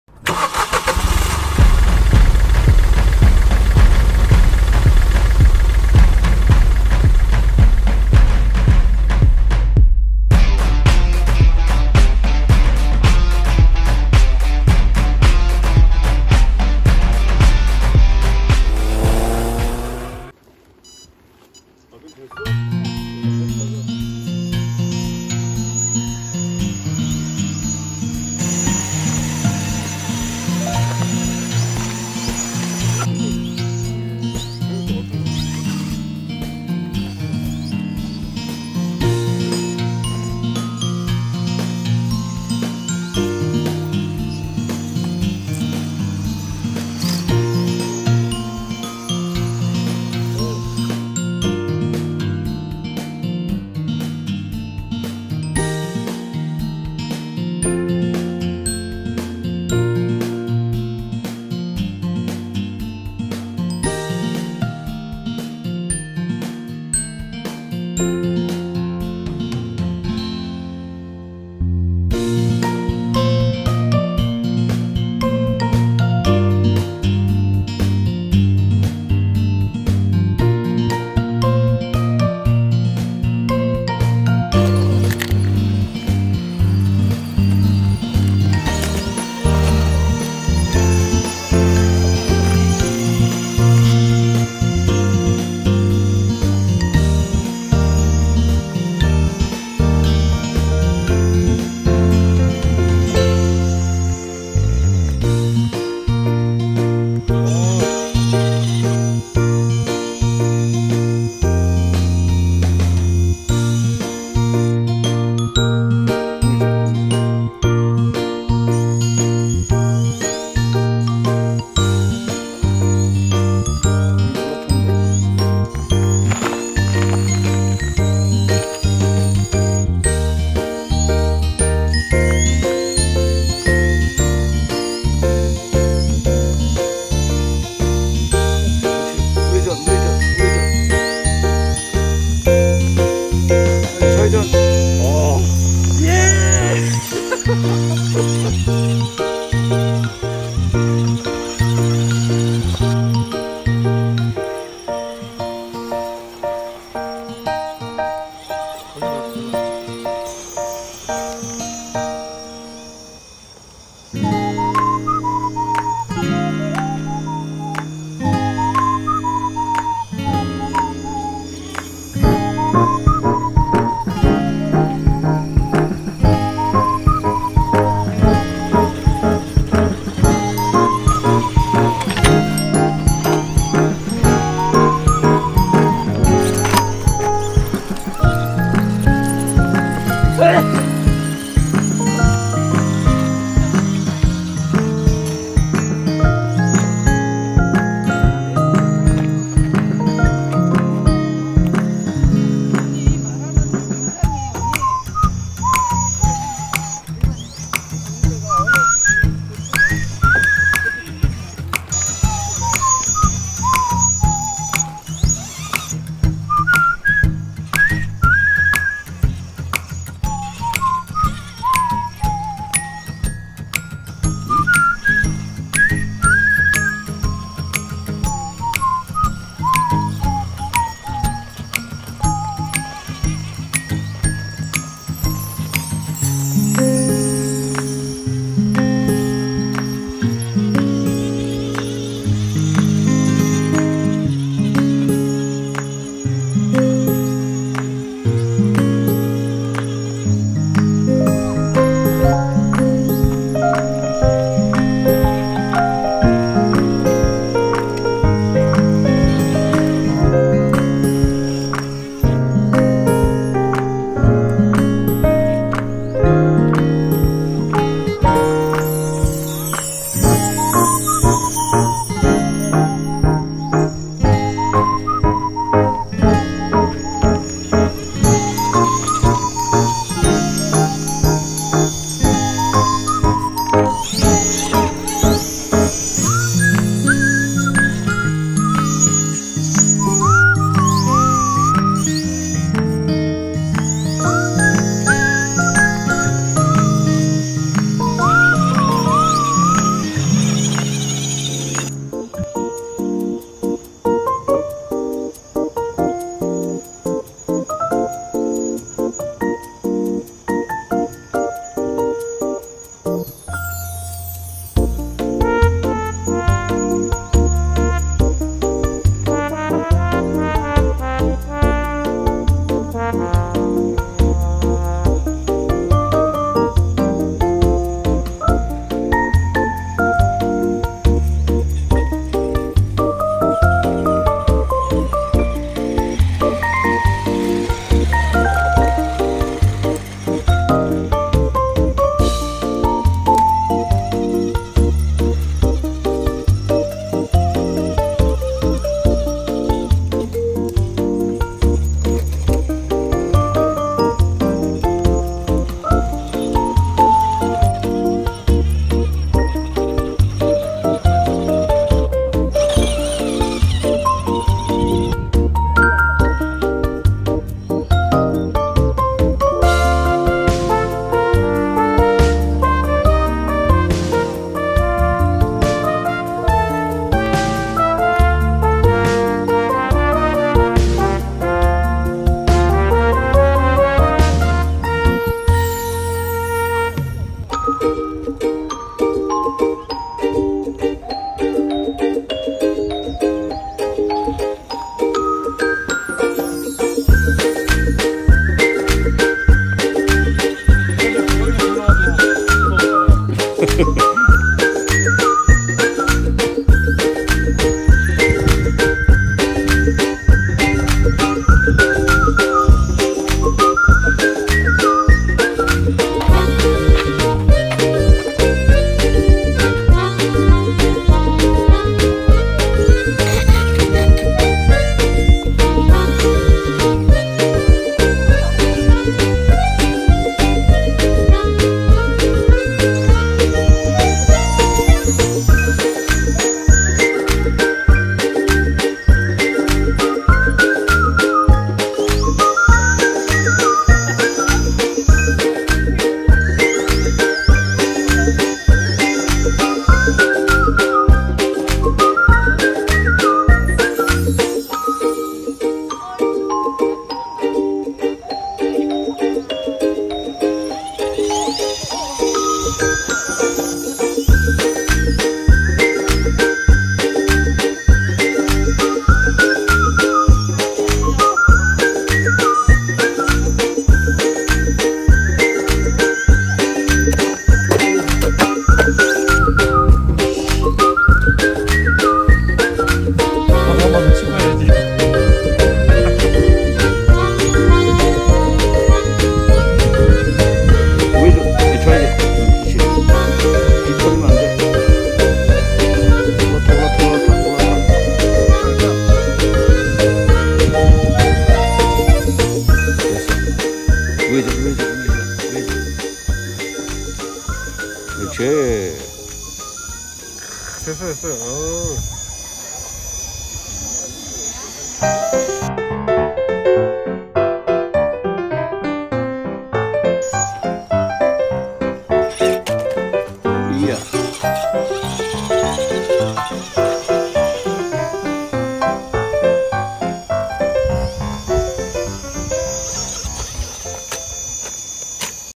Mt. Ahcha, LCG(No.4) & LFG(No.5)
With my friends at Achasan Mountain.
It's so much fun and enjoyable.

These are LCG/LFGs optimized for direct walls. It was an interesting hiking because there were various types and settings.